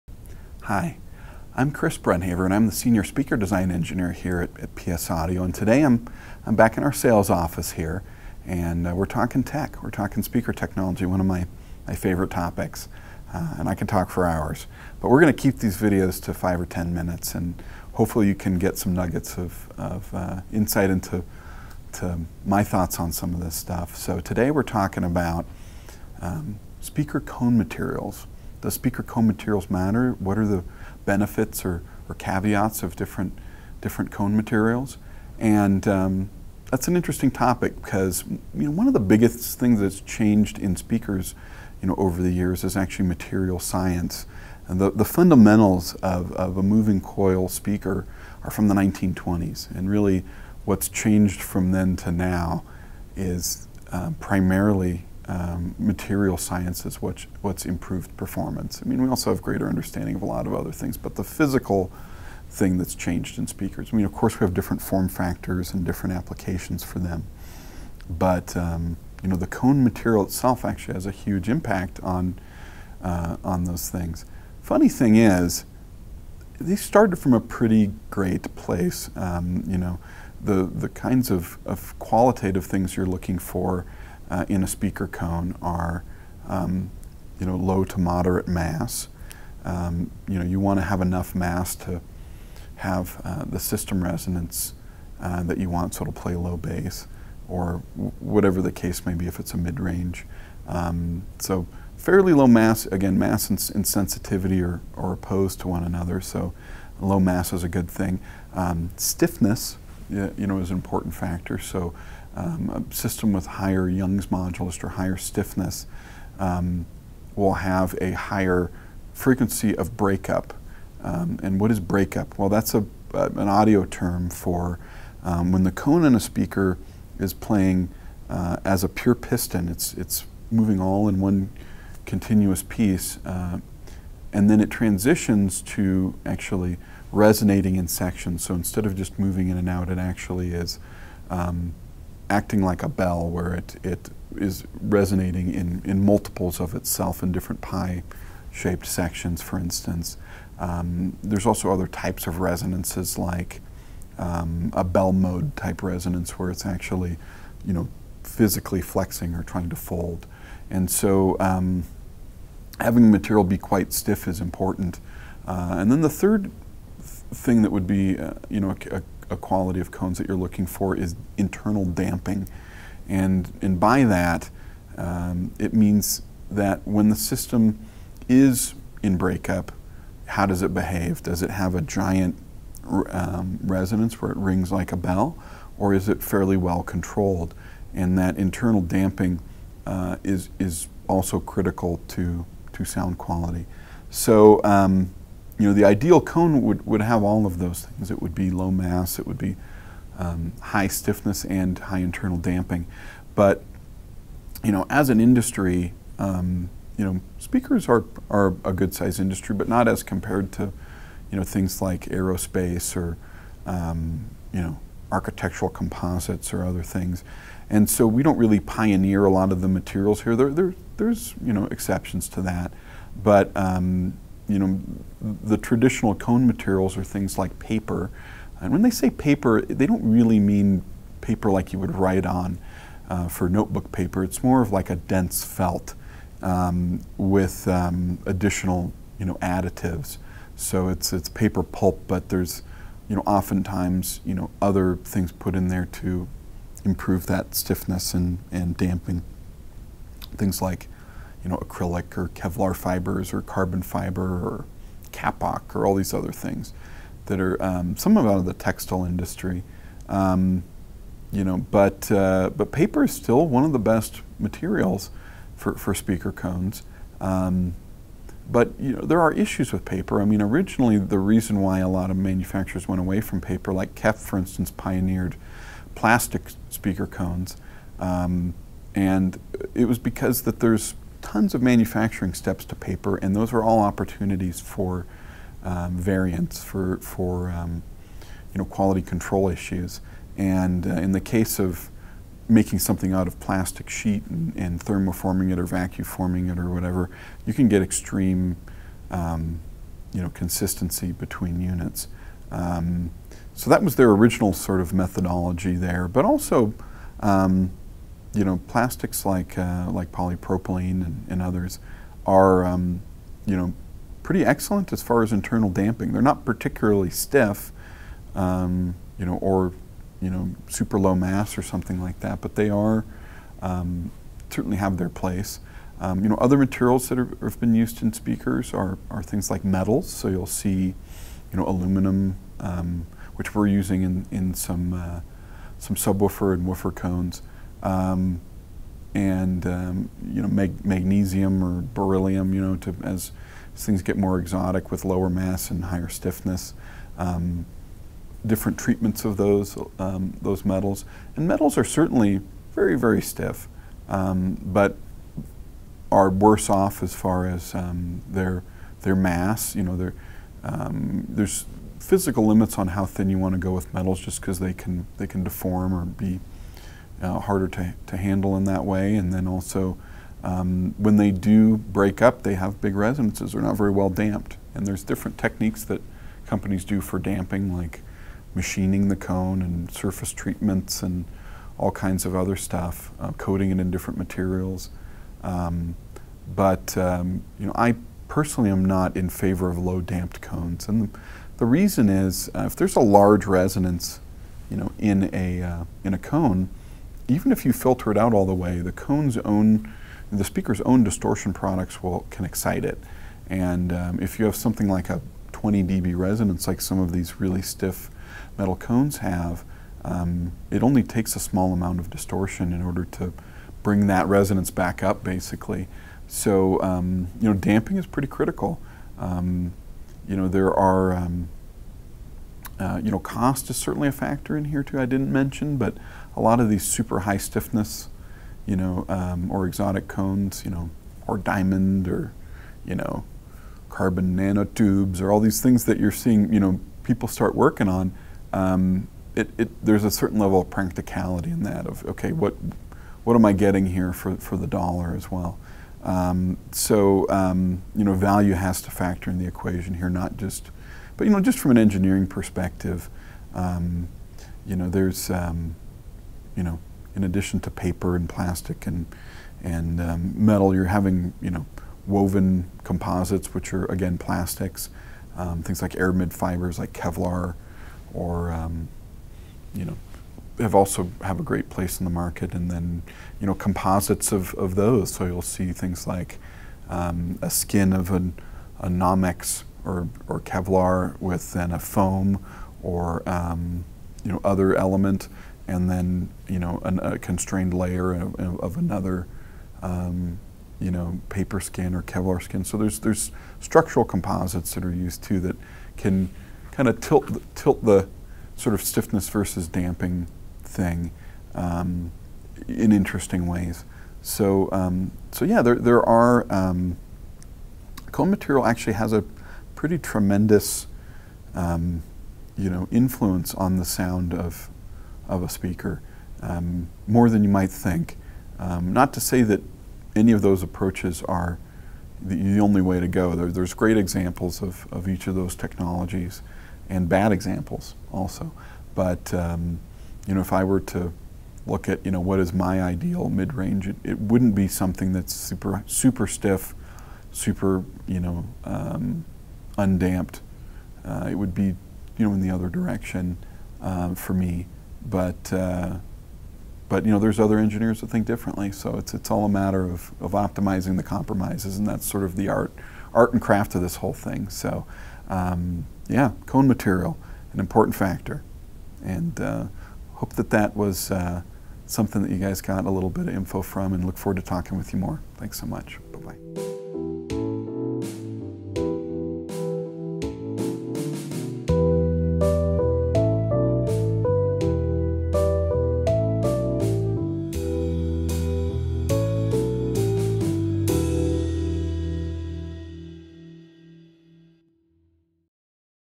Do speaker cone materials matter?
There are hundreds of speaker cone material types. Do their differences matter?